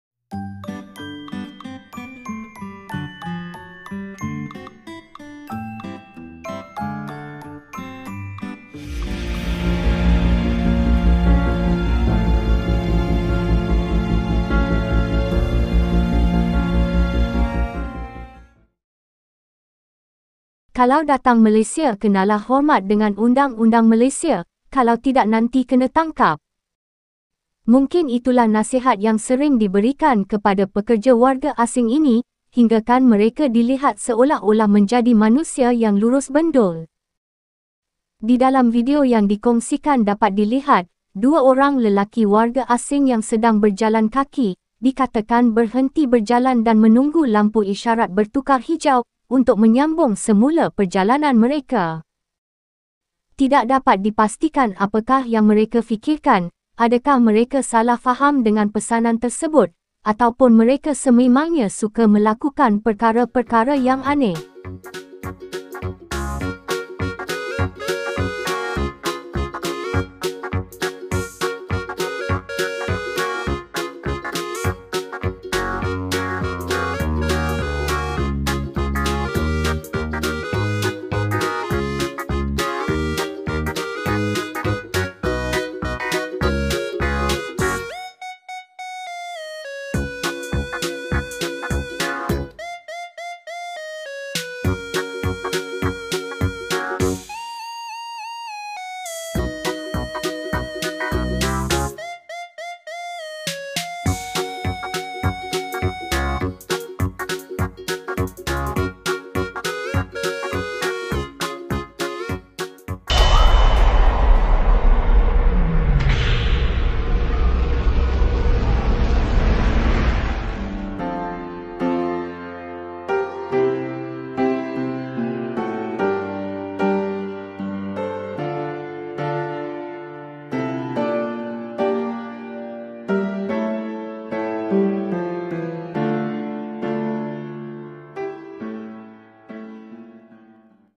KAU DAH KENAPA BERATUR MACAM KERETA
KAU DAH KENAPA BERATUR MACAM KERETA memaparkan gelagat aneh warga asing yang beratur menunggu lampu isyarat seolah-olah mereka sedang menaiki kenderaan di atas jalanraya.